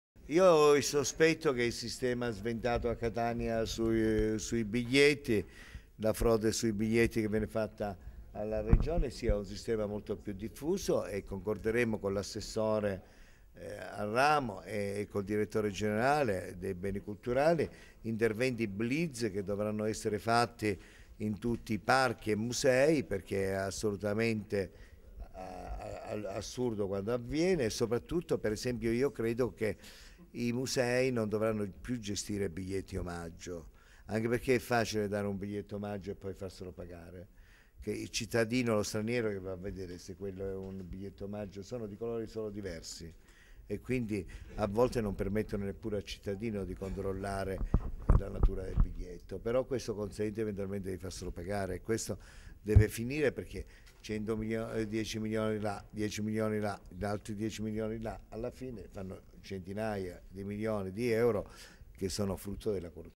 Catania, denunciato un funzionario della Regione: tratteneva gli incassi del Parco archeologico
CATANIA, 5 LUGLIO 2013 - Un funzionario della Regione siciliana è stato denunciato per aver trattenuto per sè la maggior parte degli incassi del teatro del parco archeologico greco-romano di Catania e delle aree archeologiche dei comuni vicini.